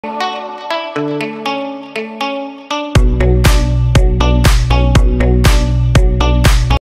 Peony Painting
Come paint these gorgeous peonies in watercolour with me!